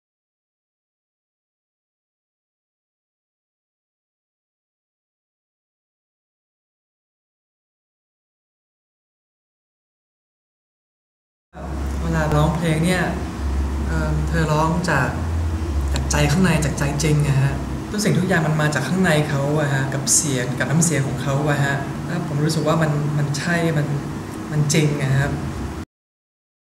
พี่น้อย วงพรู พูดถึง พี่ปาน ธนพร
XCOOLSIVE moment "PARN THANAPORN"ON MY OWN VOCAL CONCERT

คอนเสิร์ต ครั้งสมบูรณ์แบบที่สุดในชีวิตของผู้หญิง เสียงสร้างฝัน
พร้อมด้วย VOCAL รับเชิญ เบน ชลาทิศ และวง ETC.

ซื้อบัตรได้ที่ thaiticketmajor ทุกสาขาและเริ่มจำหน่ายบัตรวันแรก ในวันศุกร์ที่ 10 กันยายน 2553